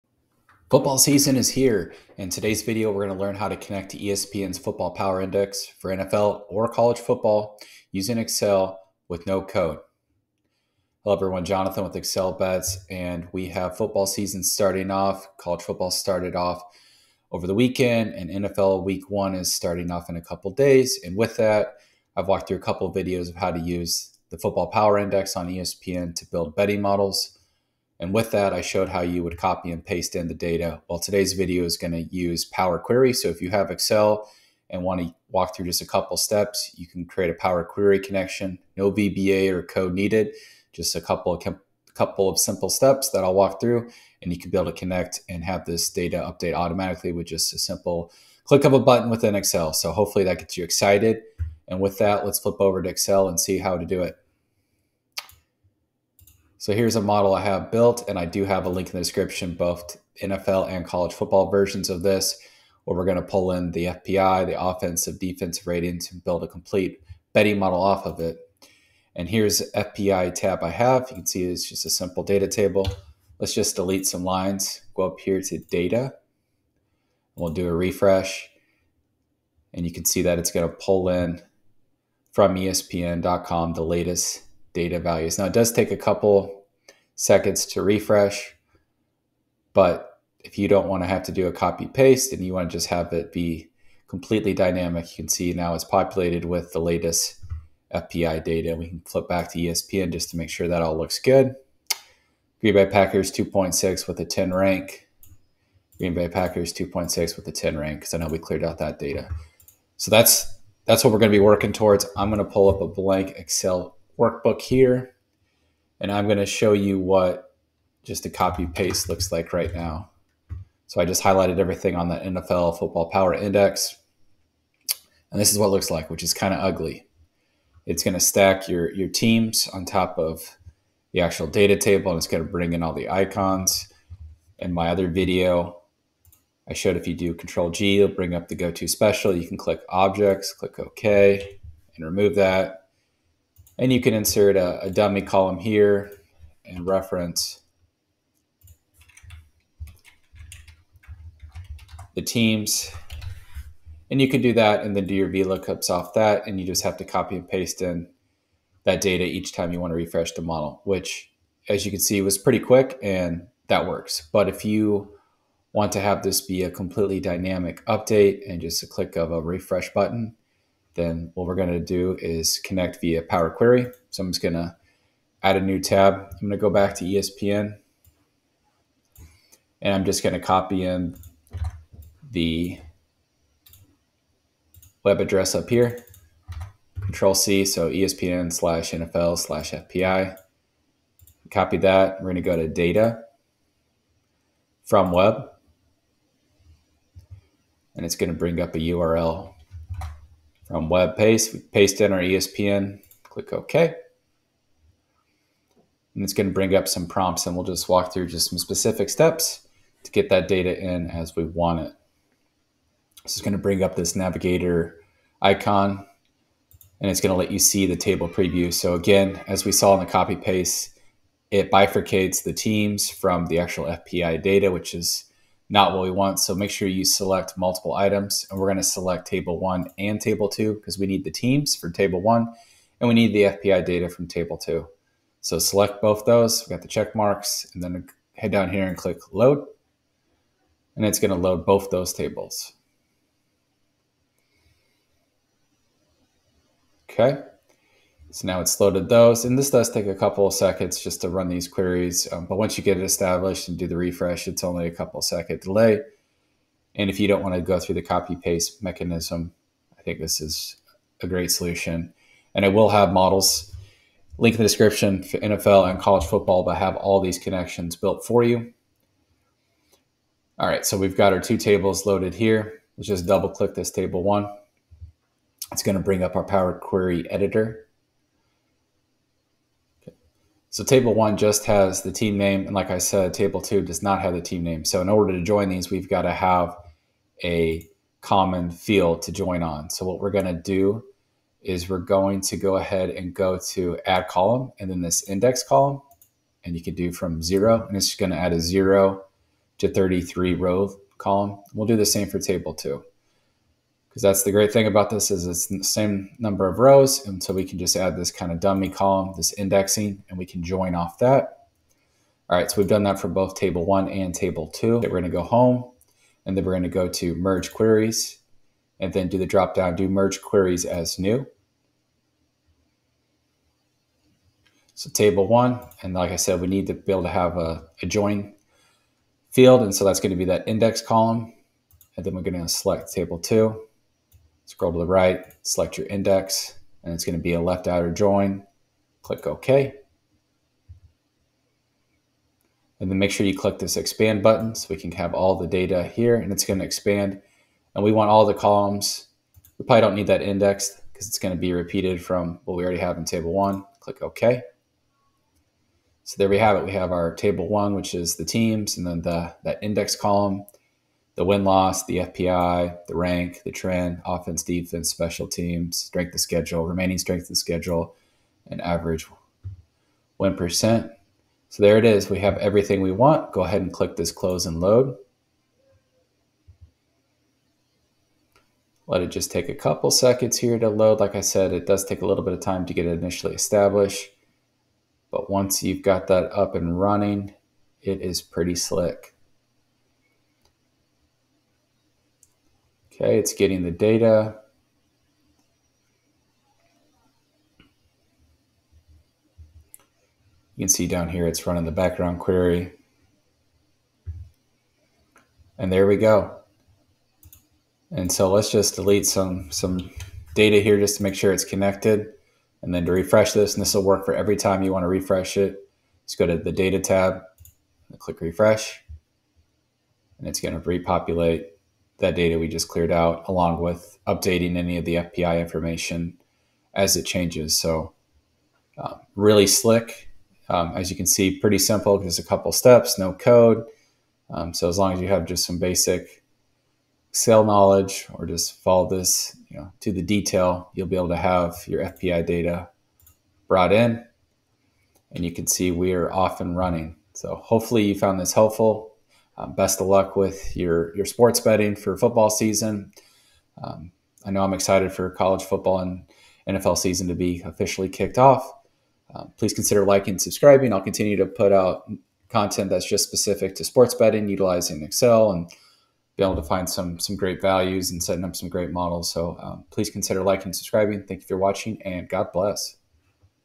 Get ESPN FPI Data Instantly with Power Query in Excel (No Code) | NFL + College Football
In this video, I am going to show you how to easily connect to ESPN FPI data in Excel.  I will walk you through the complete process of using power query in excel to create a web connection to ESPN FPI to instantly refresh NFL and College Football FPI data.

***Timestamps***
0:00 Introduction and Overview
0:55 ESPN Football Power Index (FPI) Data Connection Refresh Example
1:53 Copy and Paste Data into the Excel Model Example
2:55 Power Query Complete Steps
7:27 Conclusion and Final Thoughts

🙌 Pikkit Sports Betting Tracker | Free & Premium options for tracking your sports bets